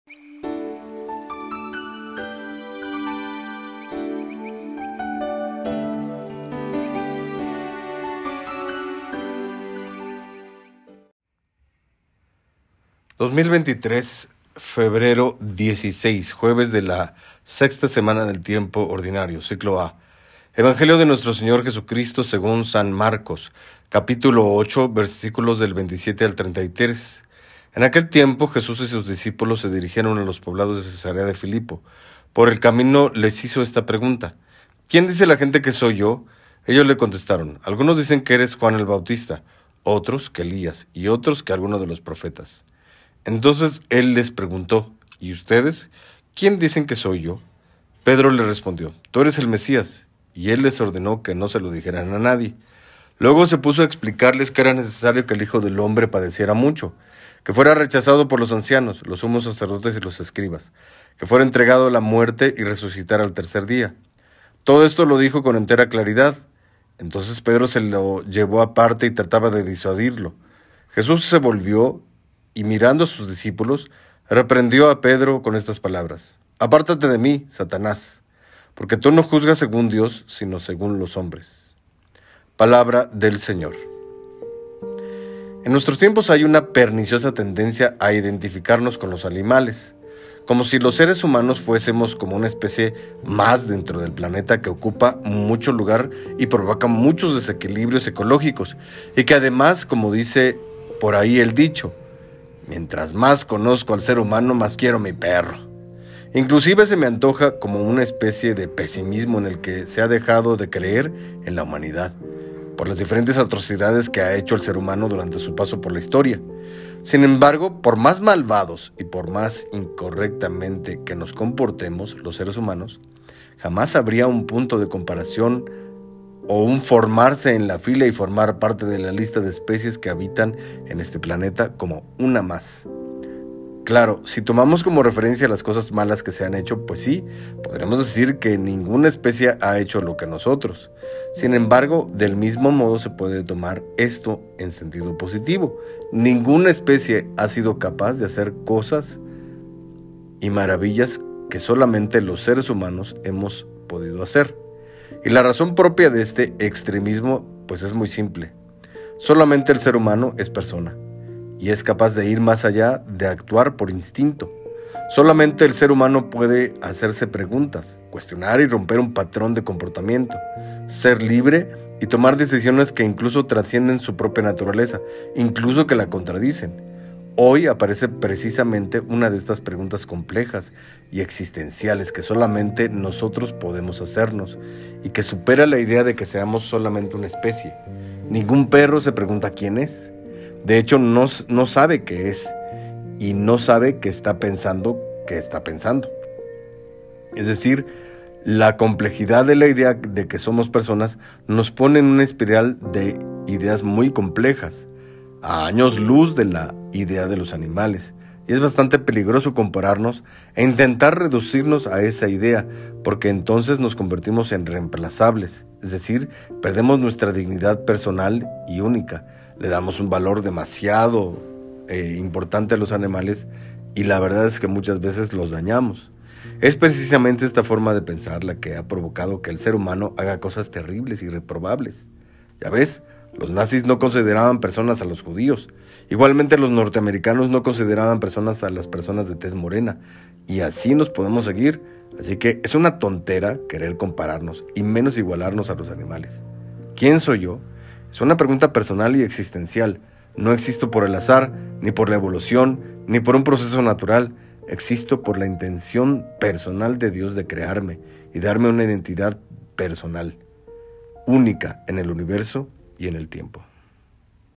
Reflexión del Evangelio jueves 16 febrero 2023, Padre Antonio Aguirre
En aquel tiempo. Jesús y sus discípulos se dirigieron a los poblados de Cesarea de Filipo. Por el camino les hizo esta pregunta...